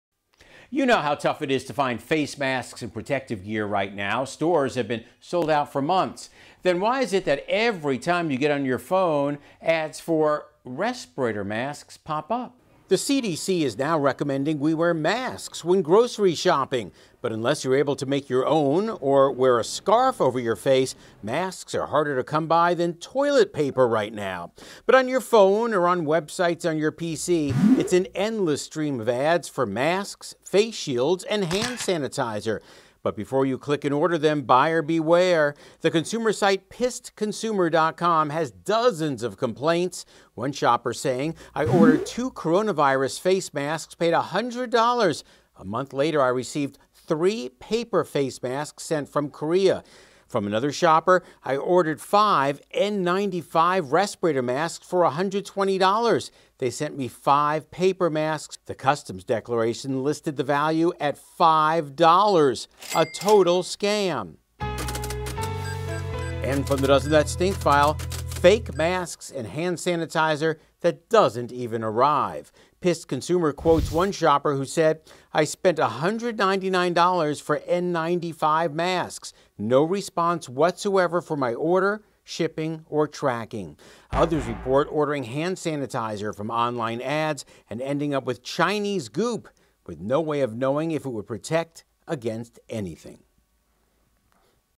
FaceMaskSellingScams
Ads are popping up everywhere for face masks, but buyer beware